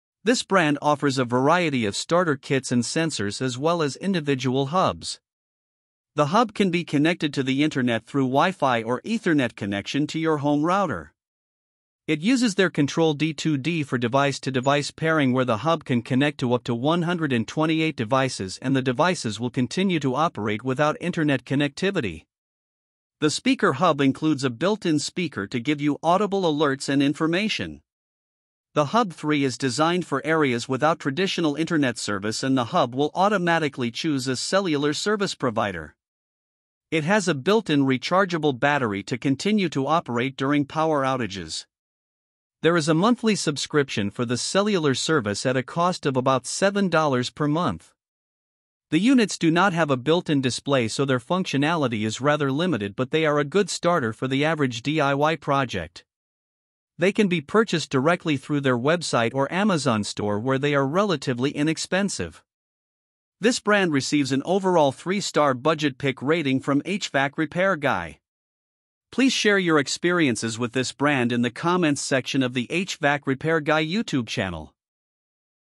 HvacRepairGuy 2024 YoLink Brand Smart Home System Reviews
In this video, HvacRepairGuy reviews the 2024 YoLink brand of smart home system hubs. These reviews are for the Hub, SpeakerHub, & Hub3 models.

You can get the reviews of all of the brands You can also compare the brands side by side using our Head To Head comparison tool.